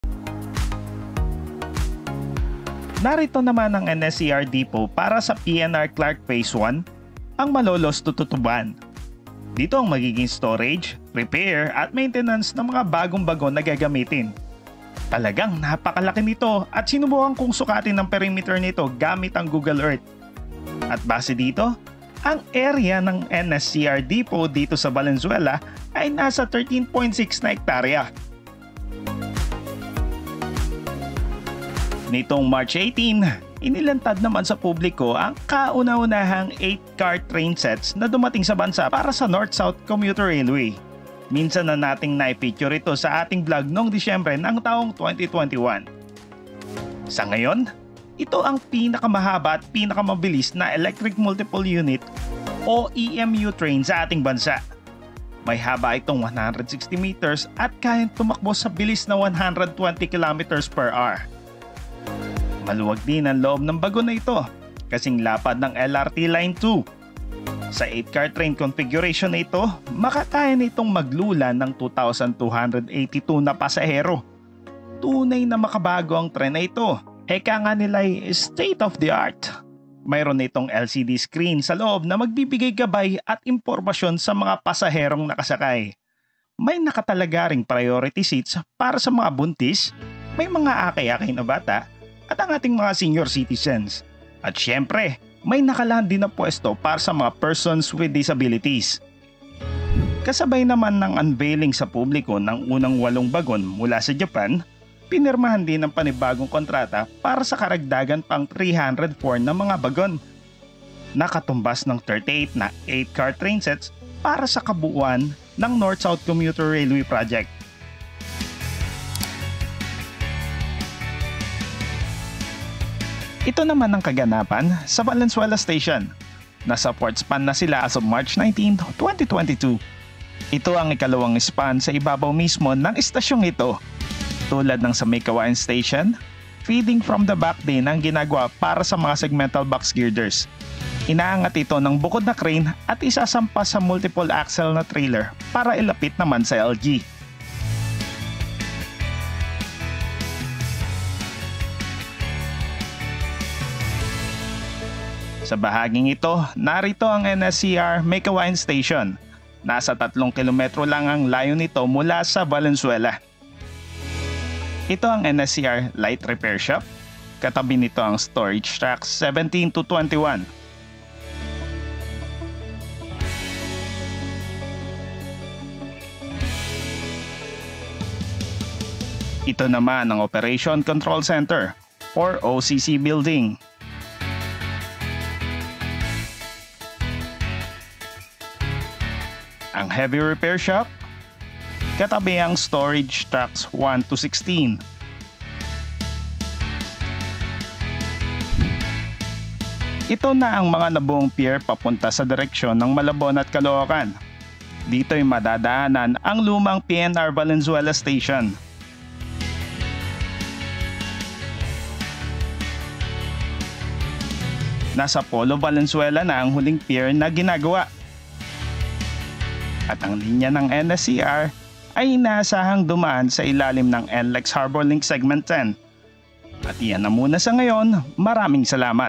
DOTr Unveils the First 8-Car Train Set | NSCR Valenzuela Depot
PNR Clark Phase 1: NSCR Depot and Valenzuela Station update as of March 19, 2022
The 8-Car trainsets from Japan arrived in the Malanday Depot last December 2021. Another Trainset is expected to come on the second quarter of 2022